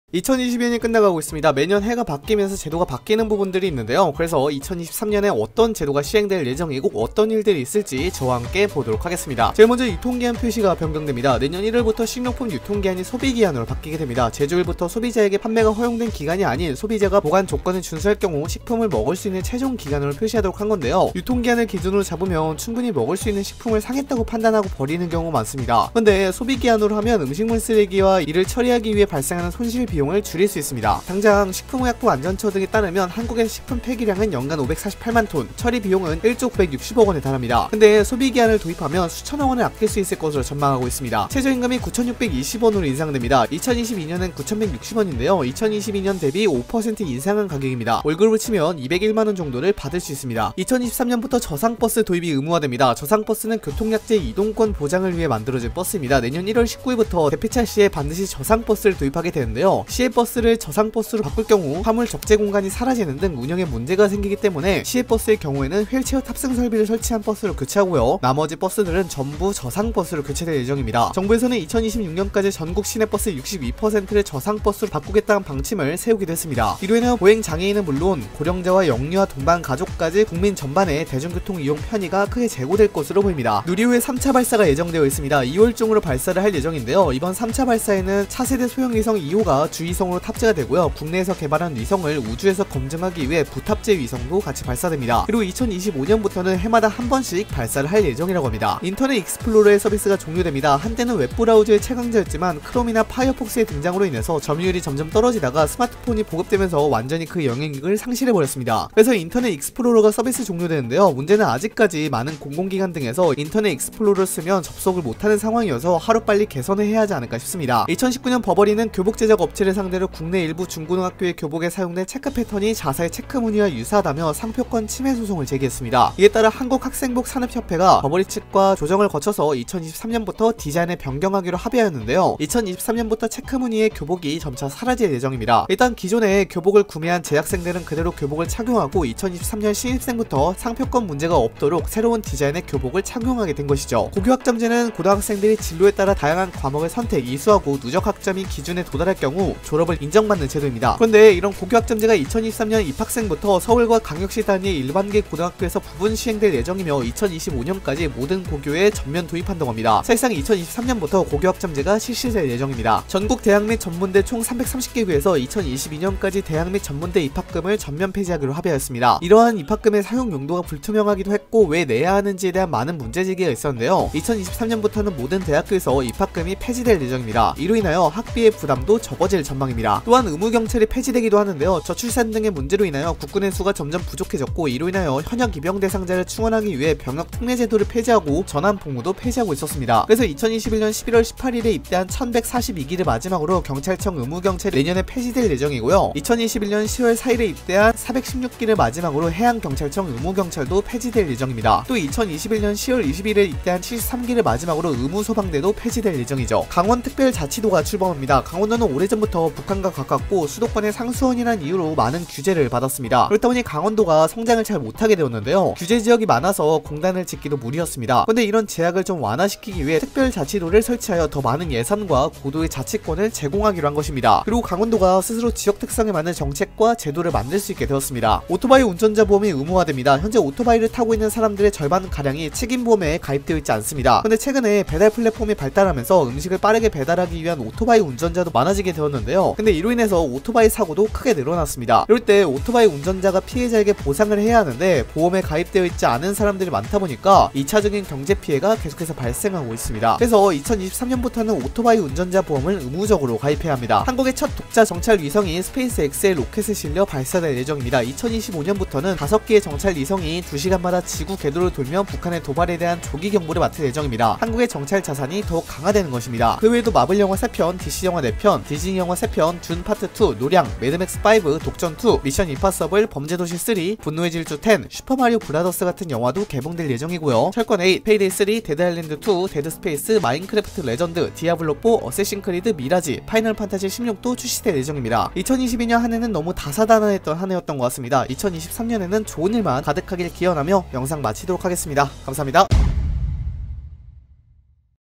사람들이 잘 모르는 2023년에 한국에서 확 바뀌는 것들! 알면 개꿀팁들!
이제 2023년입니다. 유통기한 표시제 변경, 인터넷 익스플로러 서비스 종료, 누리호 3차 발사 등 2023년에 바뀌는 것들은 무엇이 있는지 같이 볼까요?